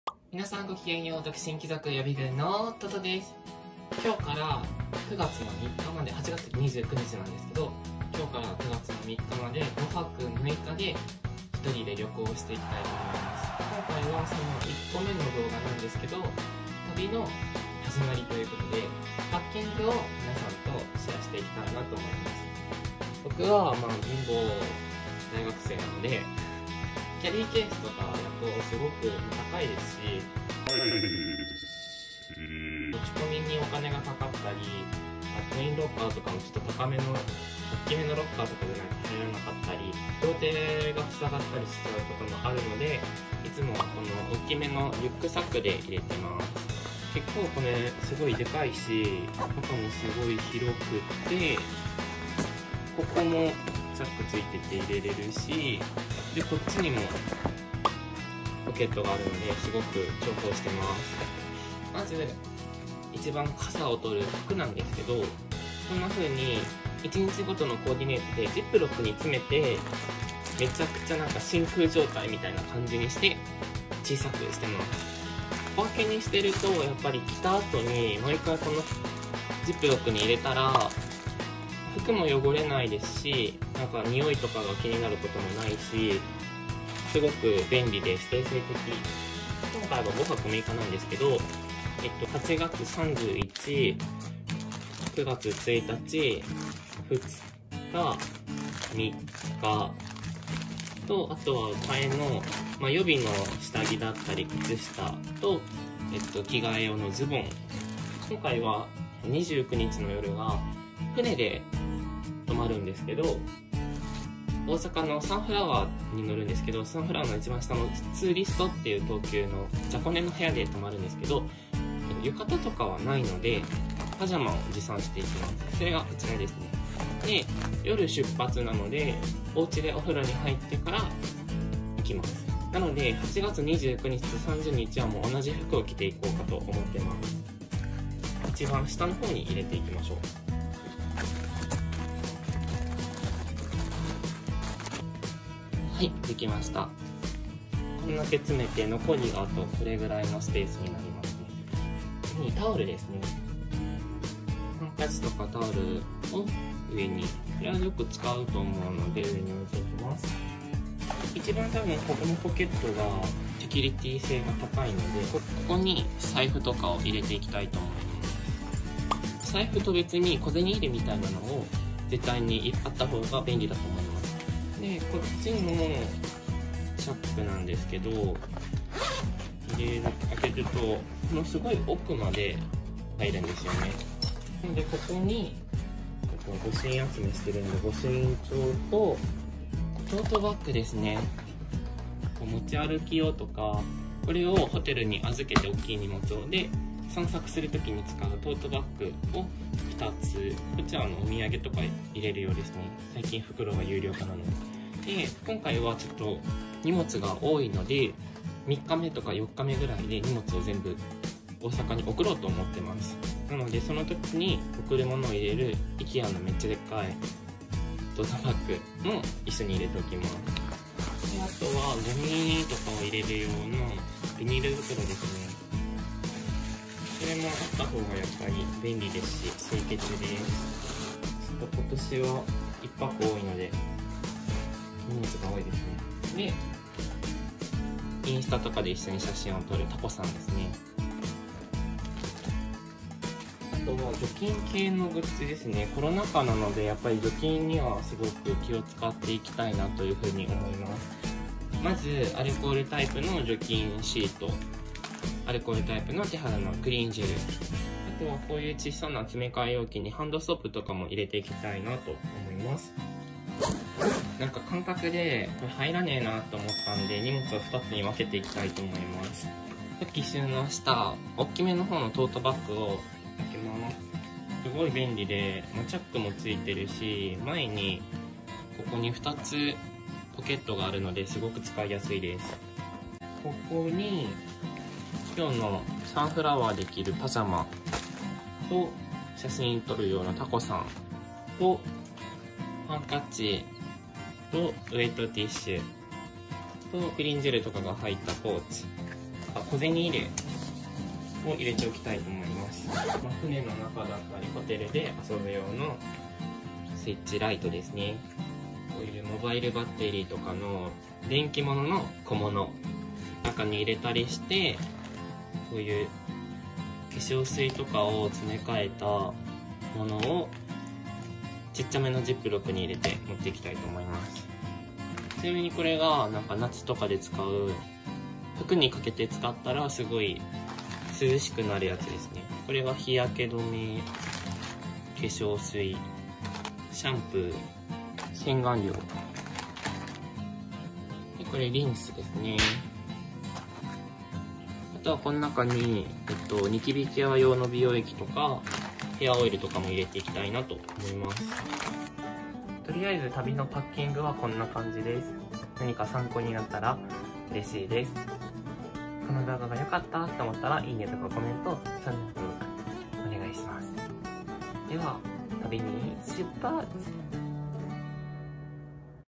【独身貴族の楽しみ】大学生1人旅のパッキング紹介！1人旅の荷物って？
こんばんは。
そして、おはようございます。
独身貴族予備軍のトトです。

今回は、今日（8/29）から始まる旅行のパッキングを紹介したいと思います！
旅行の1番の荷物は衣服！
衣服の詰め方や、かばんの使い方などをシェアしました！
ご視聴いただけたらありがたきですし嬉しみです。

トトさんのTwitter

すみません。独身貴族予備軍で調べてみてください。

ありがたさがありがたすぎて、昇天する勢いになります笑